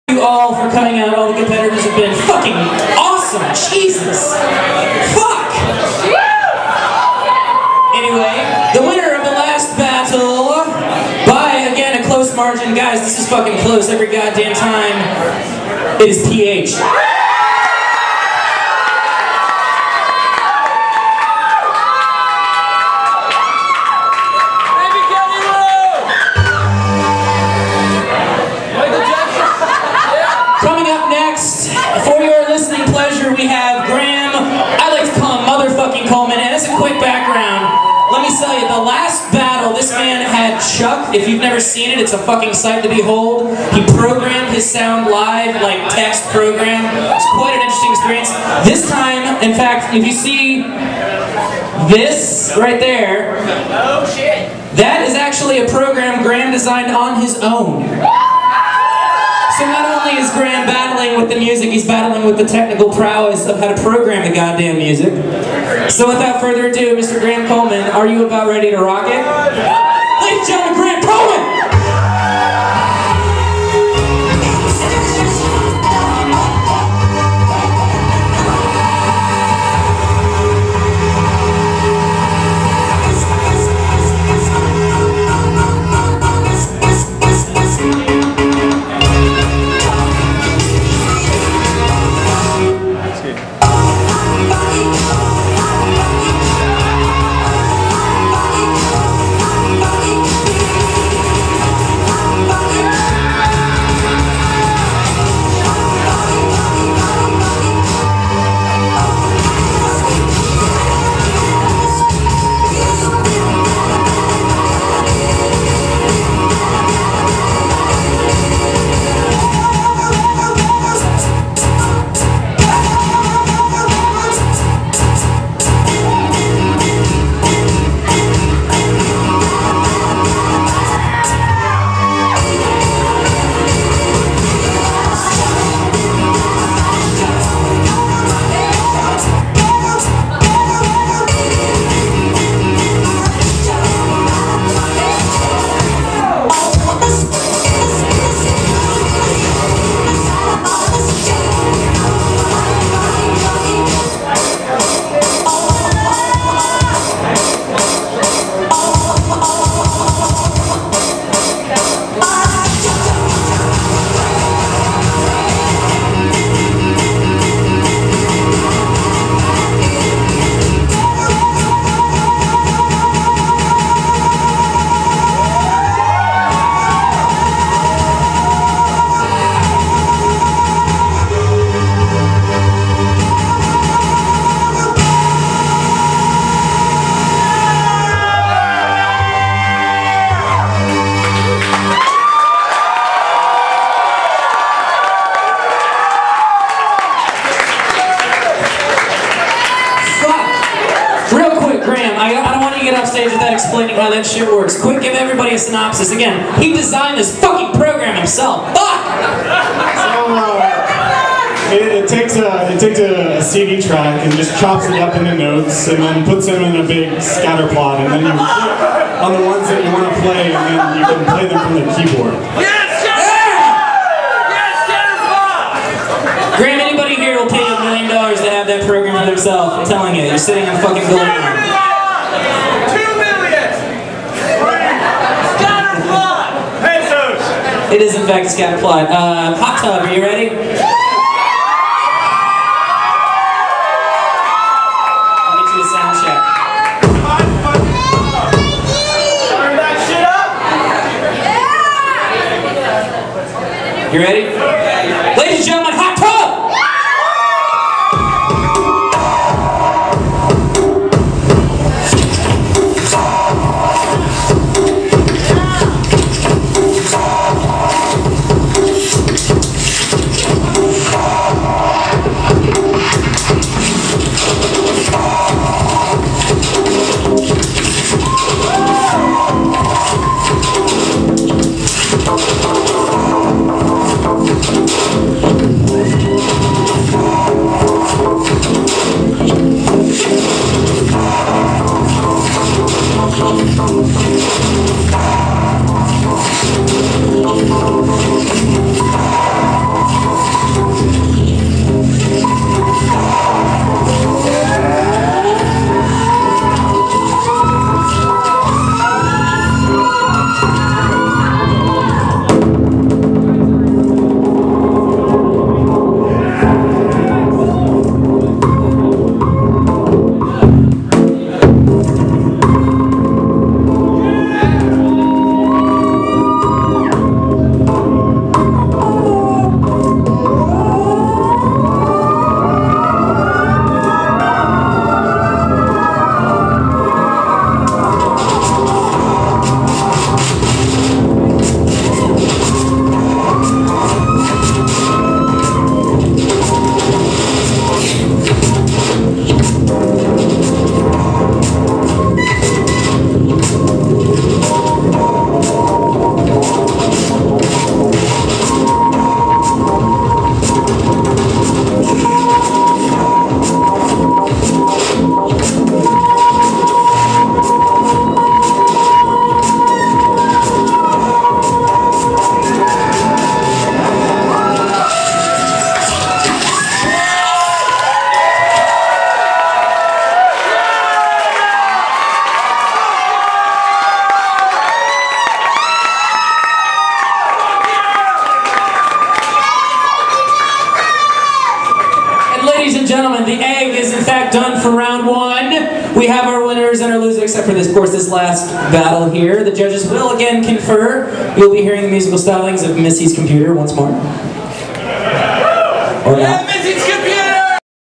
2007 Atlanta Laptop Battle, First Preliminary - Part 6 of 24
The fourth and final battle of round one brings us to Graham Coleman vs. Hottub.

datavortex
Performer: Graham Coleman
Performer: Hottub
Master of Ceremonies: Maui Threv